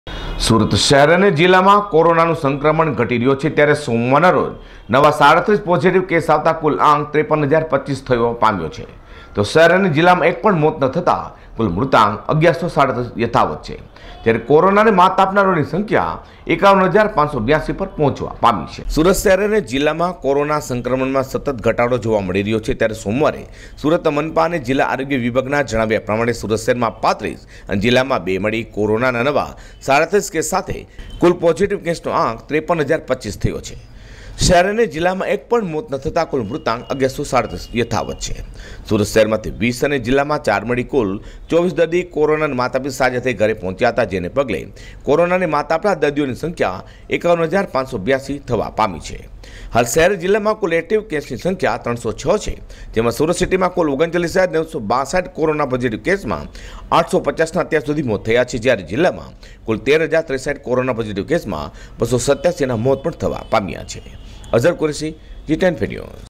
corona સુરતમાં કોરોનાના નવા 37 સામે ડિસ્ચાર્જ 24 | G 24 NEWS
સુરતમાં કોરોનાના કુલ કેસ 53,025 તો ડિસ્ચાર્જ 51,582 જ્યારે મોત 1,137